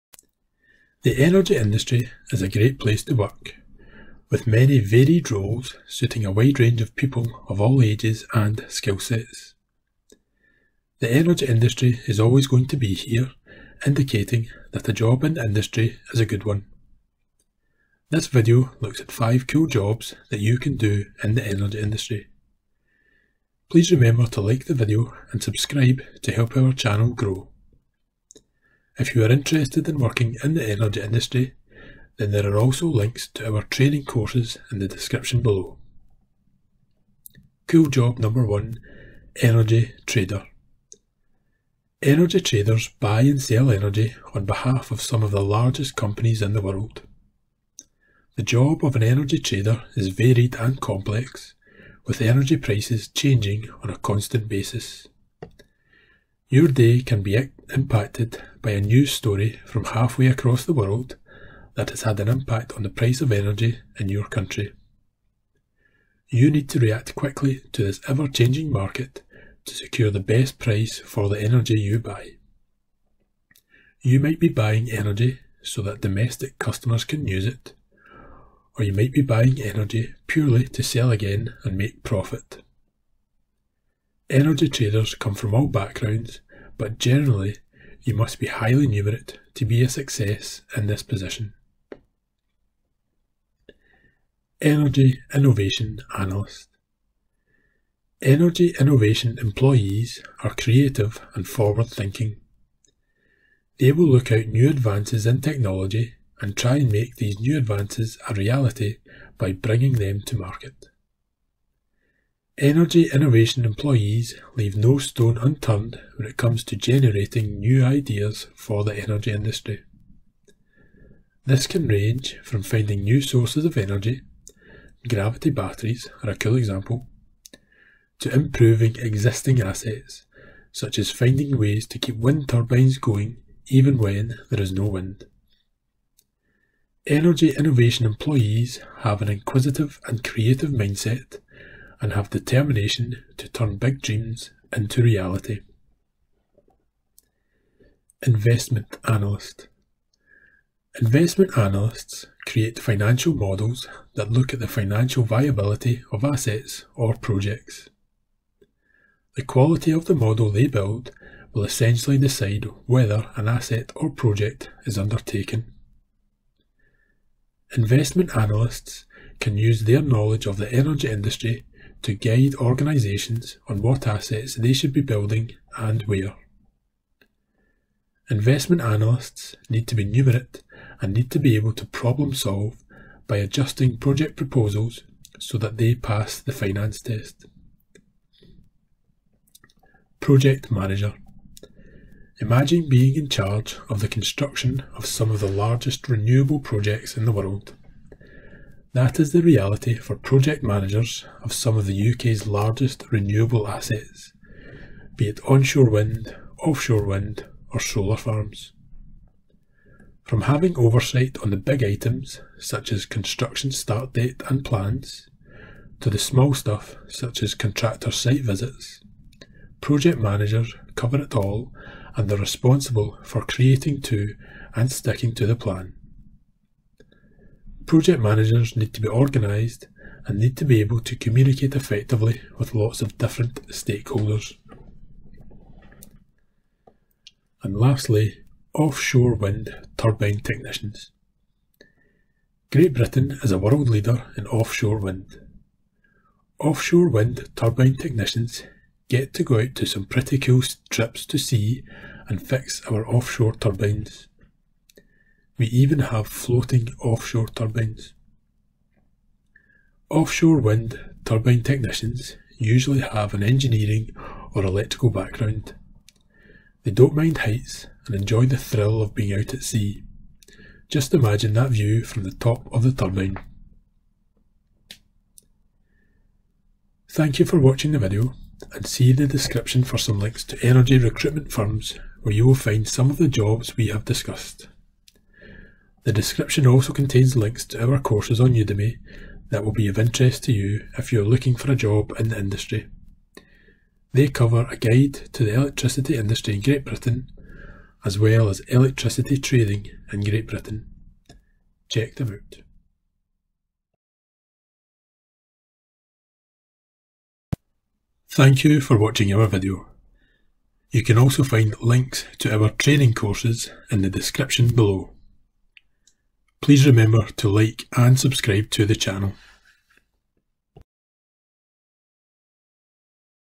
5 Cool Jobs in the Energy Industry
The energy industry is a great place to work, with many varied roles suiting a wide range of people of all ages and skill sets. The energy industry is always going to be here, indicating that a job in the industry is a good one! This video looks at 5 cool jobs that you can do in the Energy Industry. If you are interested in working in the energy industry then there are also links to our training courses in the below.

Jobs covered:
Energy Trader
Energy Innovation Analyst
Investment Analyst
Project Manager
Offshore Wind Turbine Technician

We also have a free training course, 'A Guide to The Electricity Industry in Great Britain', live on Udemy

Recruitment Agencies